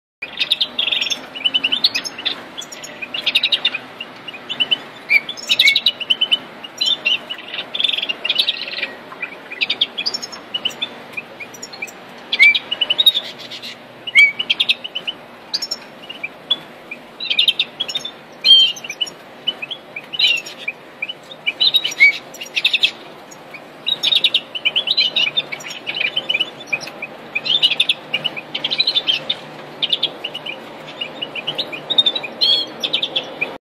Carduelis carduelis Yuva hazırlığı saka
Bilezikli eş saka yuva hazırlığı